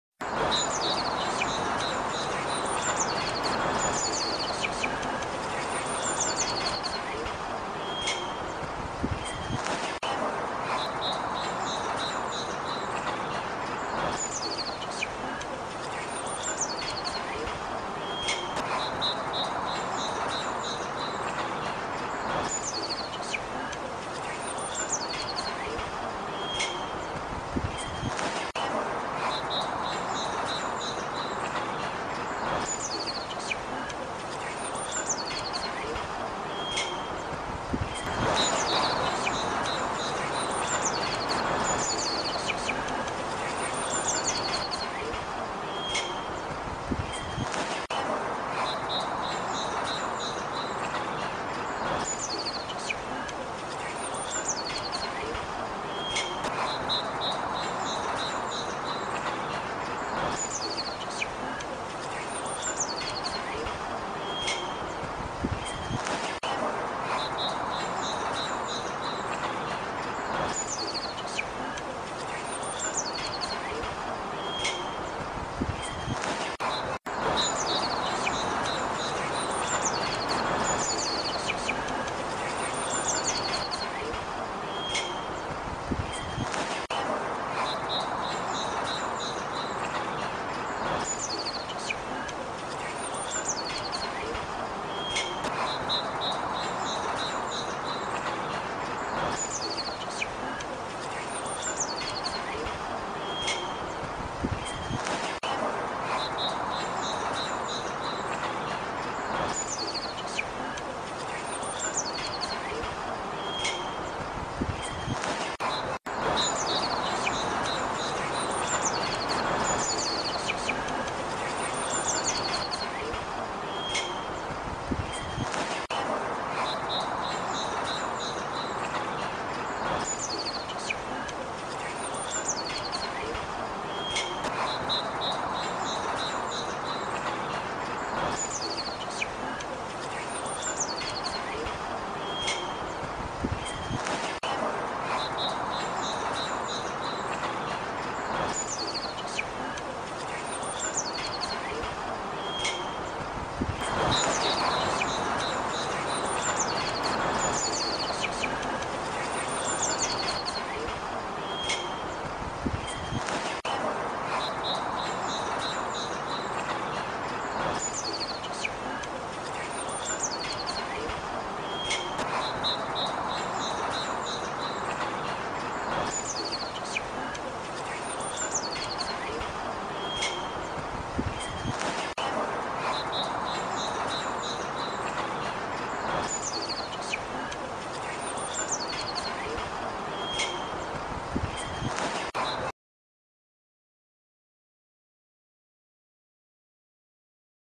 Birds Feeding - Entertain Your Cat
Birds Feeding - Entertain Your Cat.  A short video I made from filming the wildlife at a local nature reserve.